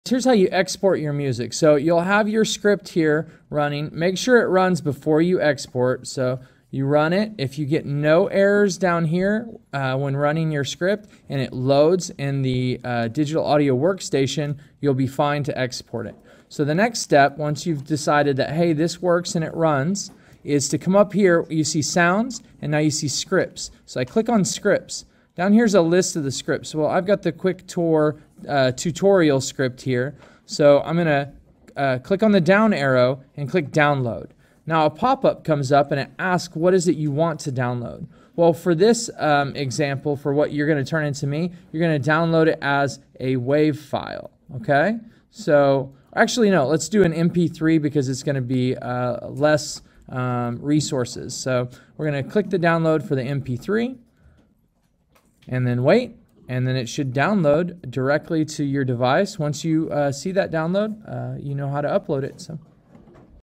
Create MP3 Ear Sketch
This video shows you how to create an MP3 through EarSketch.